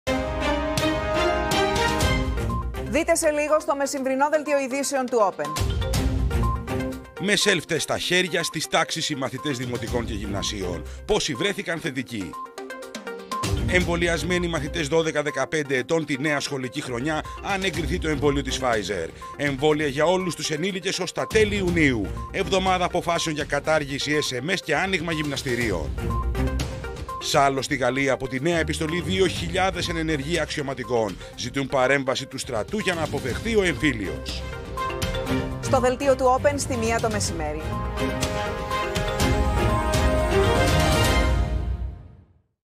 Trailer μεσημβρινού δελτίου ειδήσεων 10/05/2021 | OPEN TV
Δείτε στο δελτίο ειδήσεων του OPEN στις 13:00 με τη Λίνα Δρούγκα: Mε self test στα χέρια, στις τάξεις οι μαθητές δημοτικών και γυμνάσιων. Πόσοι βρέθηκαν θετικοί. Εμβολιασμένοι οι μαθητές 12-15 ετών τη νέα σχολική χρονιά, αν εγκριθεί το εμβόλιο της Pfizer. Εμβόλια για όλους τους ενήλικες ως τα τέλη Ιουνίου. Εβδομάδα αποφάσεων για κατάργηση sms και άνοιγμα γυμναστηρίων. Σάλος στη Γαλλία από τη νέα επιστολή 2.000 εν ενεργεία αξιωματικών. Ζητούν παρέμβαση του στρατού για να αποφευχθεί ο εμφύλιος.